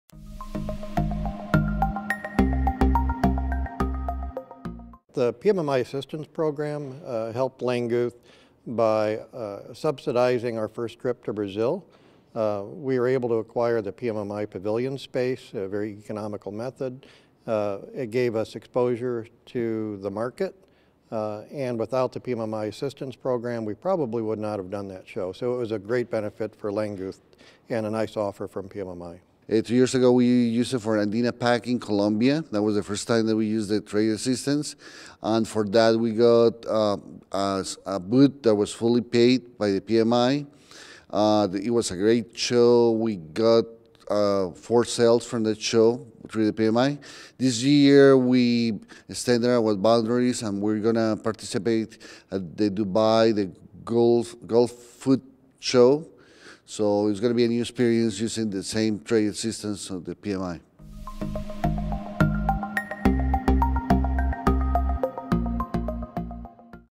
PMMI Trade Show Assistance Makes Reaching New Markets Possible 1
Learn how other PMMI member companies have used our International Tradeshow Assistance Program in this new video from PMMI Global Marketing.